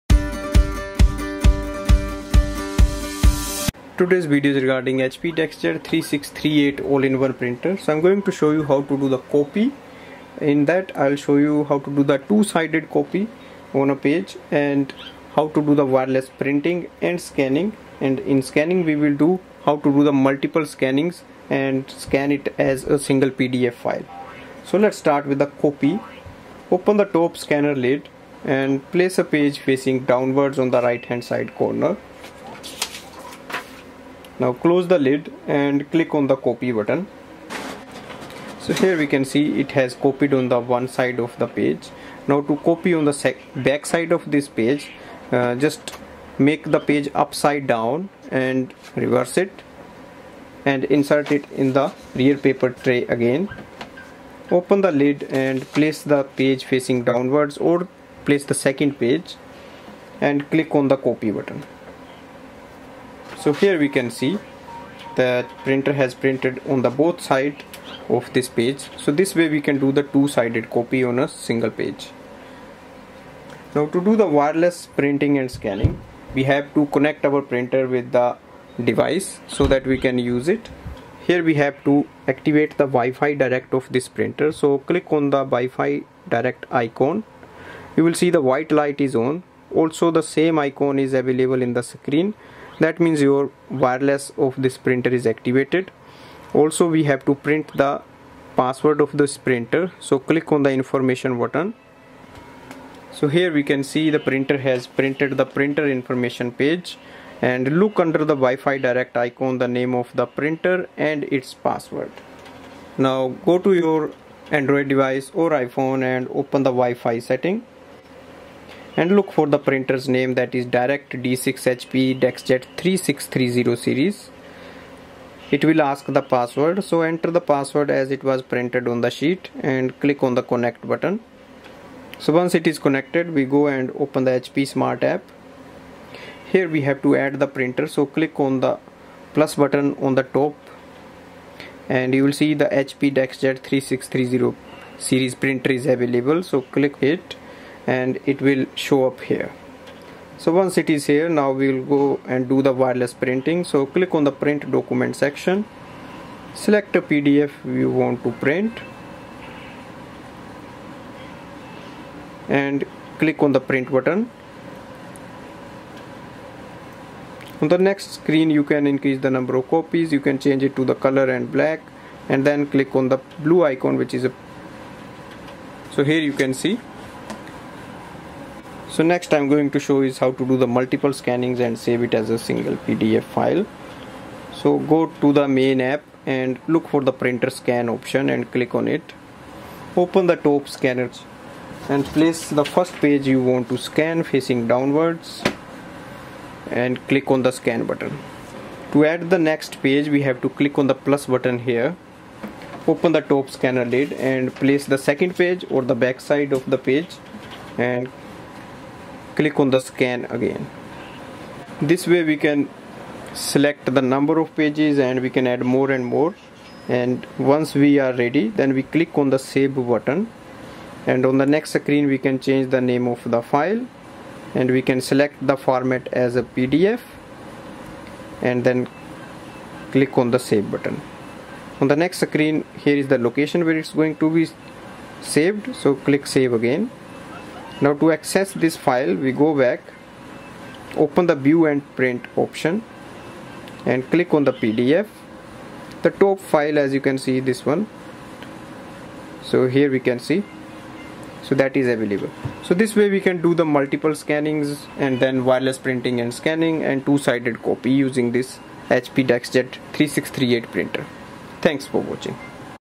How to Copy, Print & Scan with HP DeskJet 3638 All-in-one Printer ?
This video tutorial reviews the step-by-step method to do 2-sided copy, wireless printing & scanning of multiple pages/ documents as a single PDF file with your HP DeskJet 3638 All-in-one Printer. You learn to use wireless wifi direct for wireless use with an Android phone with HP Smart App.